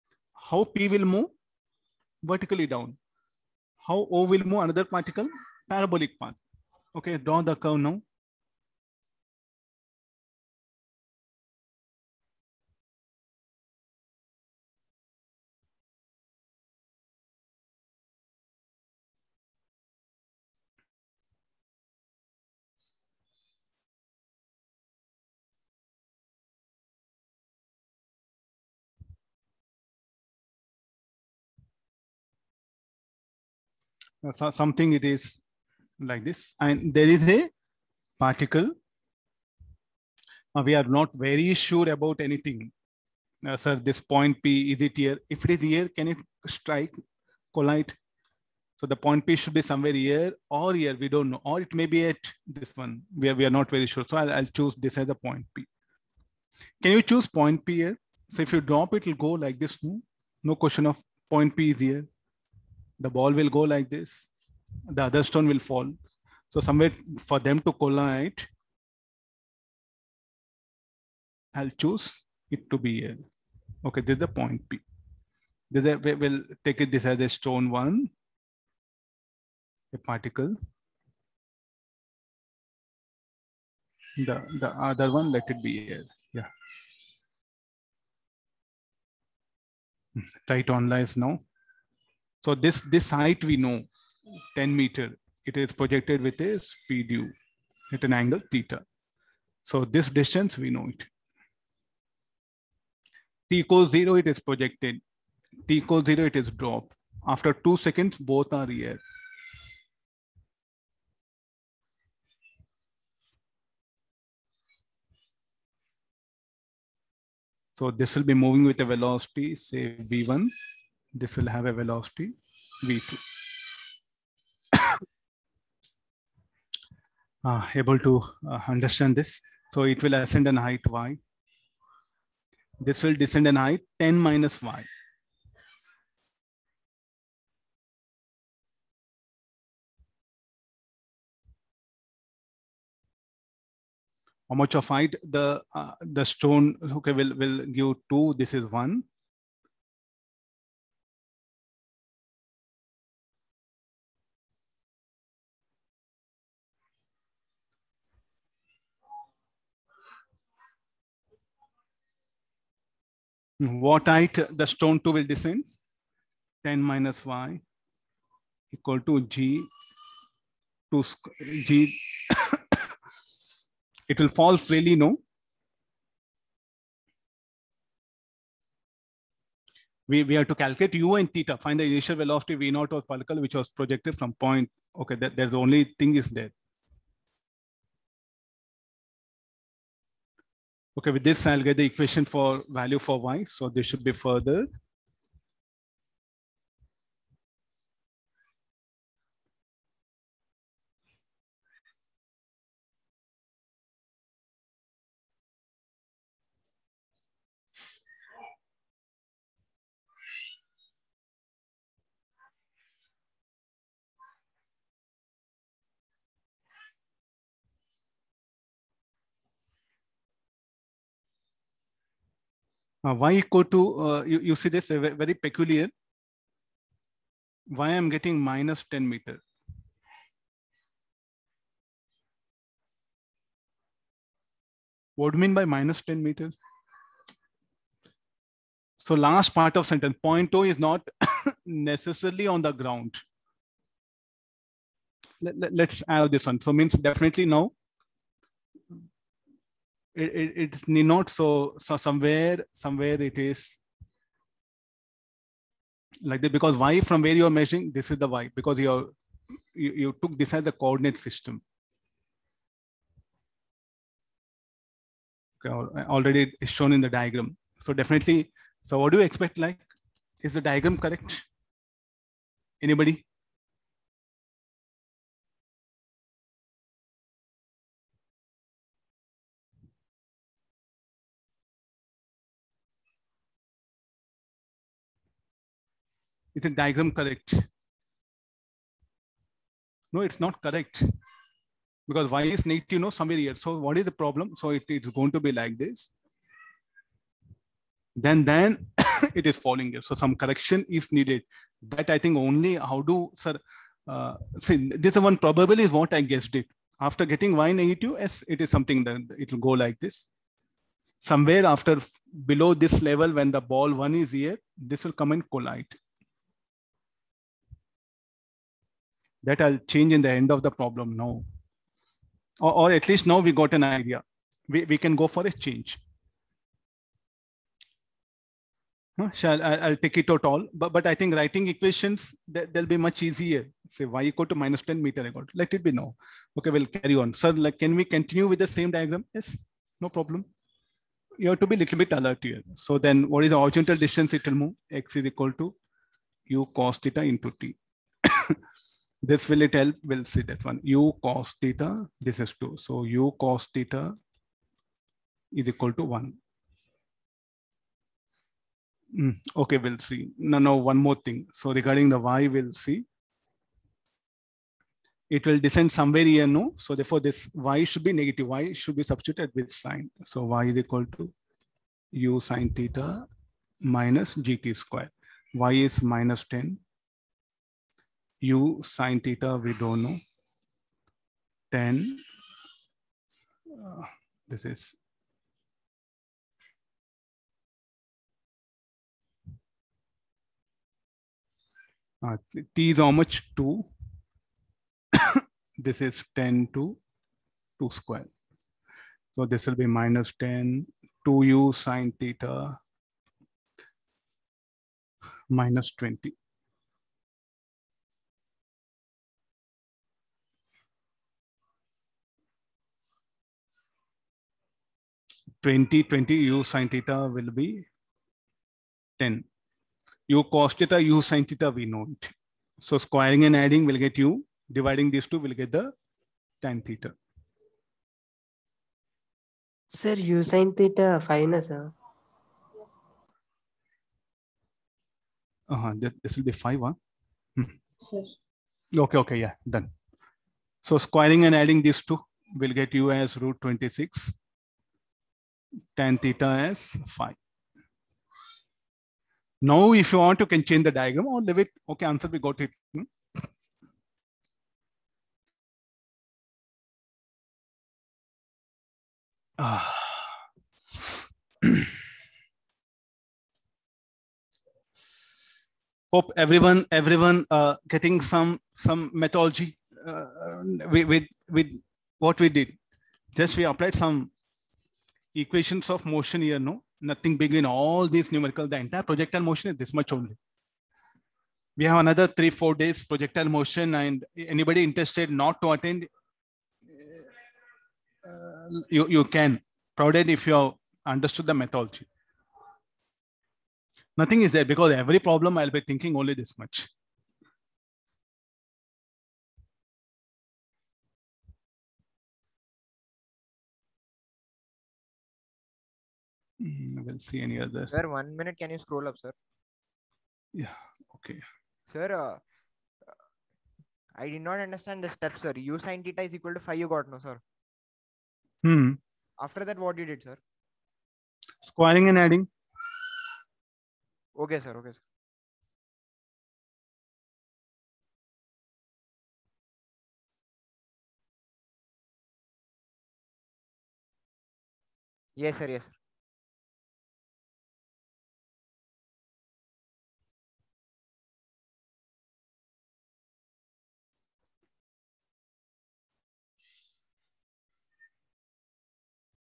24) Projectile Motion | A particle is dropped from point P at time t = 0. At the same time another
particle is thrown from point O as shown in the figure and it collides with the particle P. Acceleration due to gravity is along the negative y-axis. If the two particles collide 2 s after they start, find the initial velocity v 0 � 0 of the particle which was projected from O. Point O is not necessarily on ground.